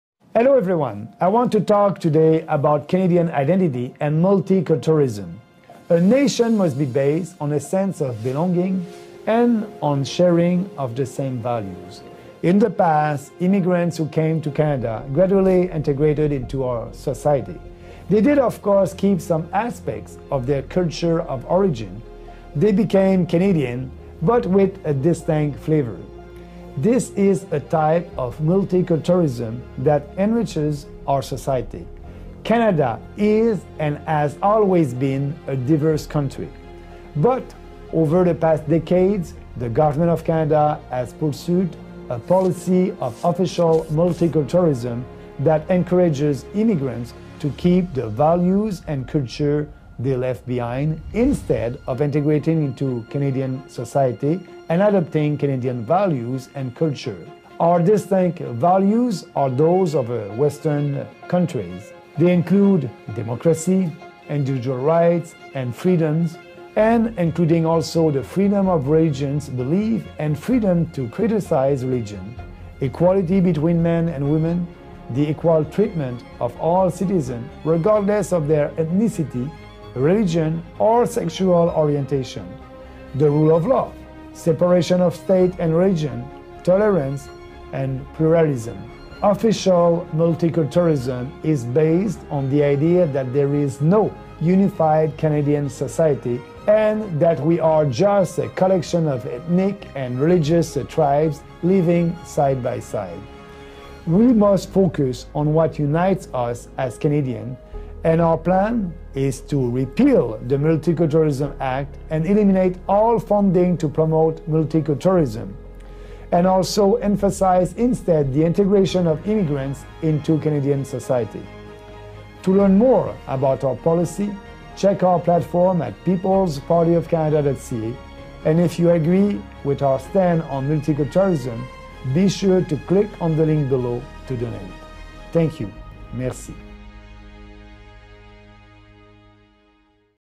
Canadian Identity And Multiculturalism
"Official multiculturalism is based on the idea that there is no unified Canadian society and no distinct Canadian identity to integrate into, and that we are just a collection of ethnic and religious tribes living side by side." Maxime Bernier